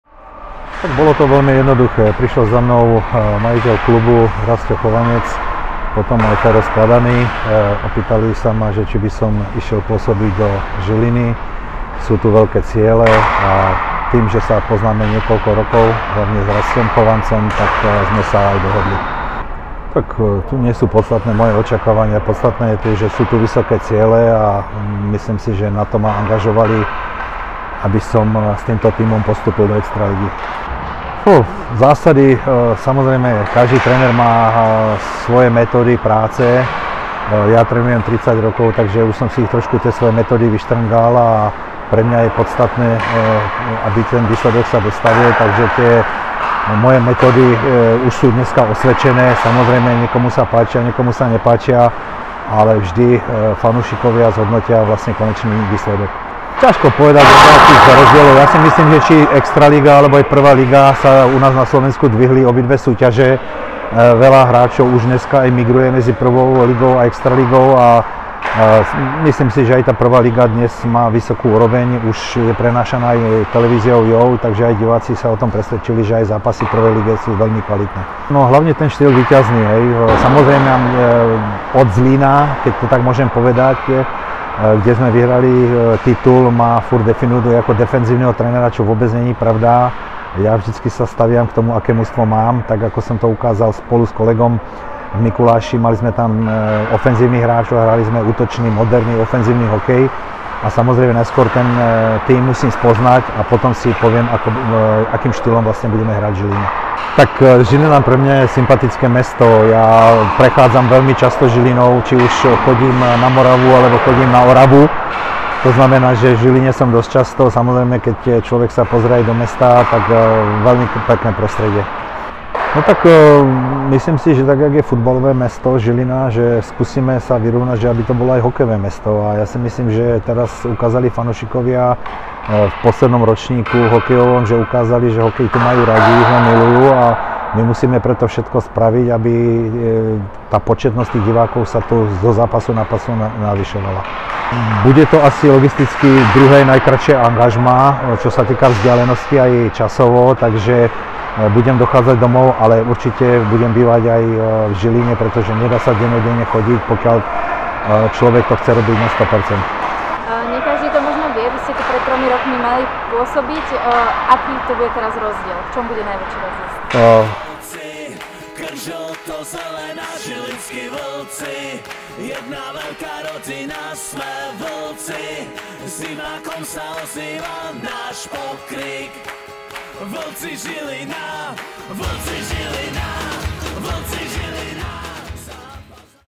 Hlavný tréner Vlkov v sezóne 2023/2024: Ernest Bokroš
Našu svorku v novej sezóne Tipos SHL povedie skúsený slovenský tréner Ernest Bokroš. Naposledy pôsobil ako hlavný tréner Liptovského MIkuláša. Za sebou má bohaté skúsenosti zo slovenskej extraligy, českej extraligy či aj z medzinárodného fóra. Má za sebou mnoho úspechov ako napríklad zlato z Majstrovstiev sveta v roku 2002 či striebro z 2000. Dlhé roky stál na lavičke slovenskej reprezentácie do 20 rokov, odkiaľ s našimi chlapcami taktiež doniesli cenný kov (bronz). Zaujímavosťou je aj titul v českej extralige s klubom HC Zlín. Inými slovami, tréner Bokroš má za sebou bohatú trénerskú kariéru.

Vitaj medzi Vlkmi.